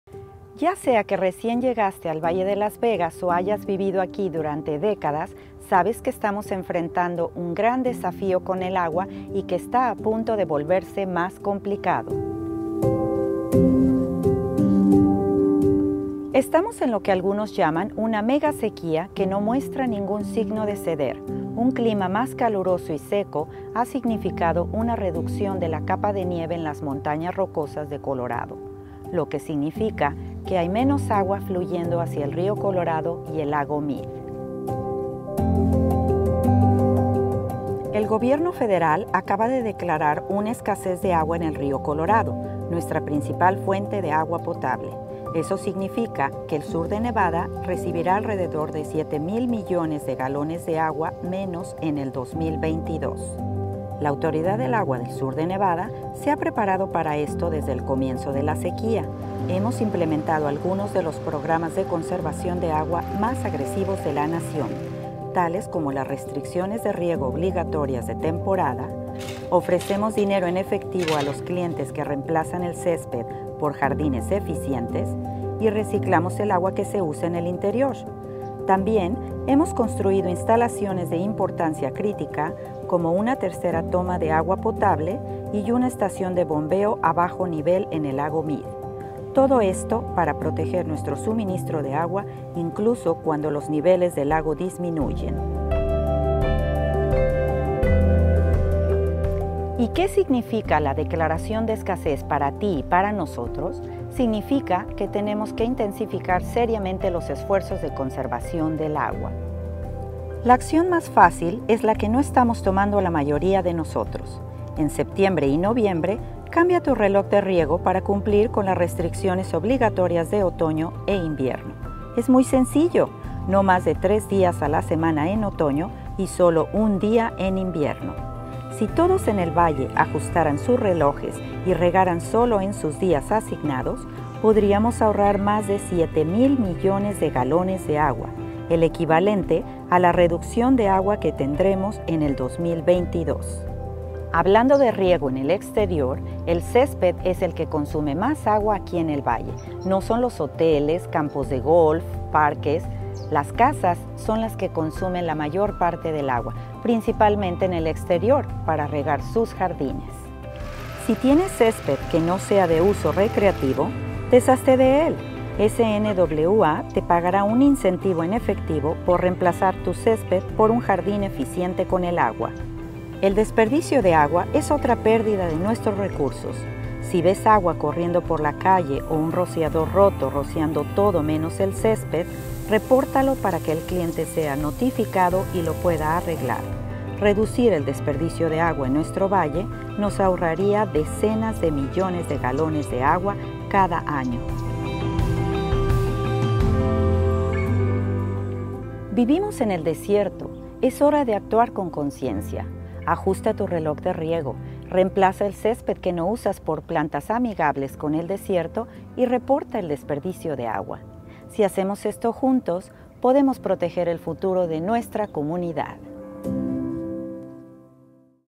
¿Qué significa para Las Vegas la escasez de agua federal en el río Colorado?
Nos enfrentamos a grandes desafíos relacionados con el agua en nuestro valle y están a punto de volverse más complicados. El gobierno federal ha declarado una escasez de agua en el río Colorado, nuestra principal fuente de agua potable. Adriana Picazo explica lo que esto significa para el sur de Nevada.